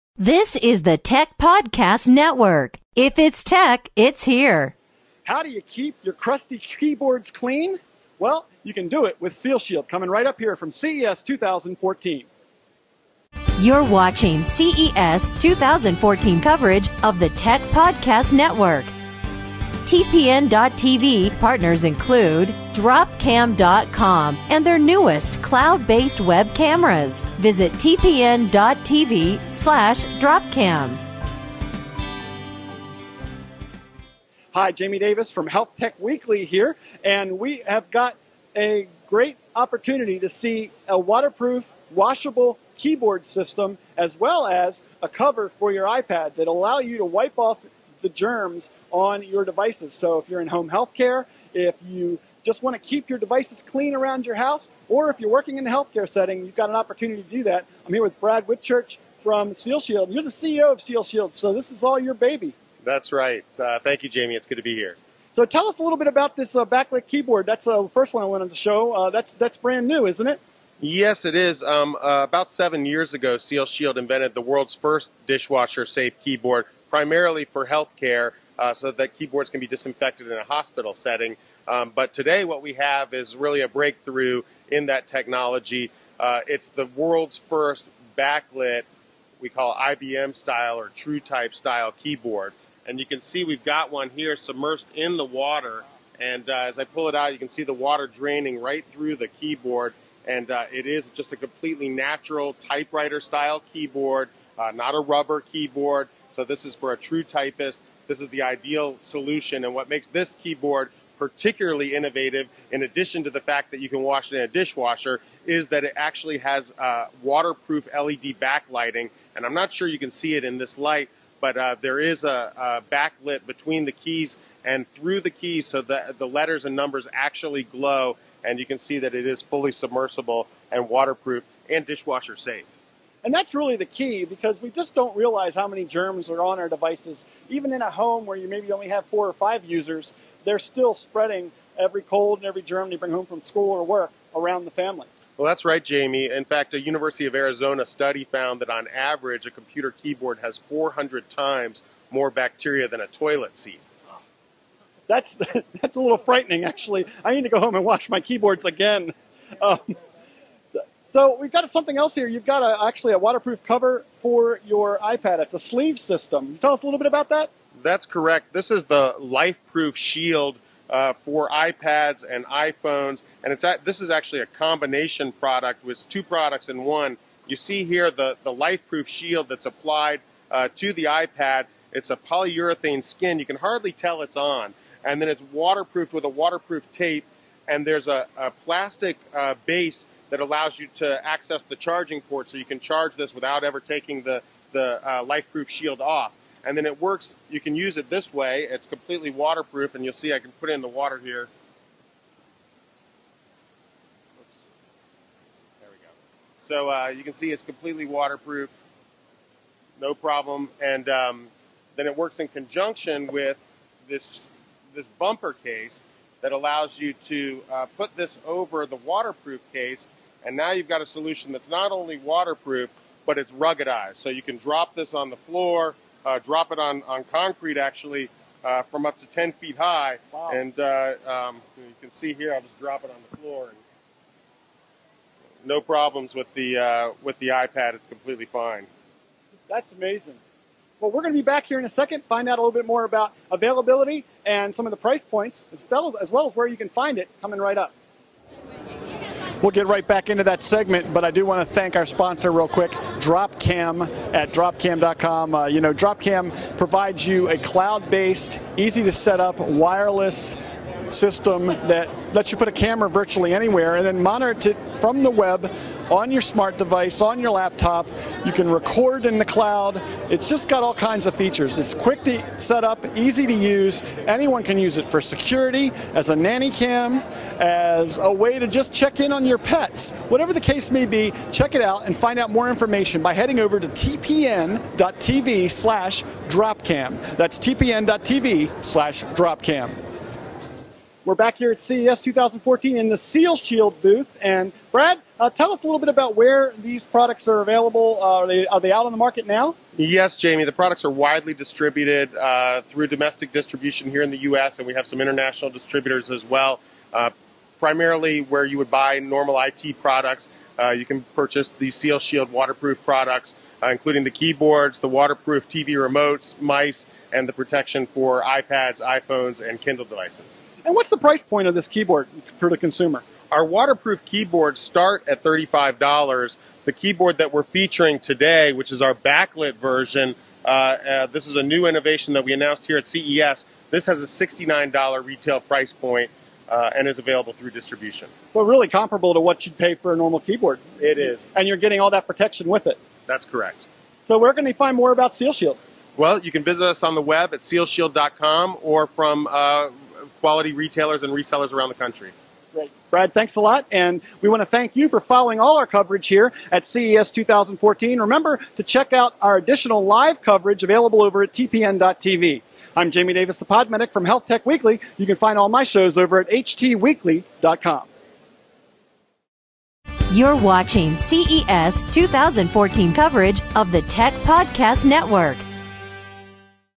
Sealshield Computer Keyboards Control Infection in the Dishwasher
Did you know your keyboards at home and at work are the dirtiest places in our lives? SealShield knows and they've created the best solution out there, a completely washable keyboard line that's actually dishwasher safe! Health Tech Weekly host Jamie Davis, the Podmedic got the chance to chat with SealShield CEO Brad Whitchurch in the digital health pavilion at the International CES conference this year about their brand new products for the office and home. Find more segments like this at HTWeekly.com.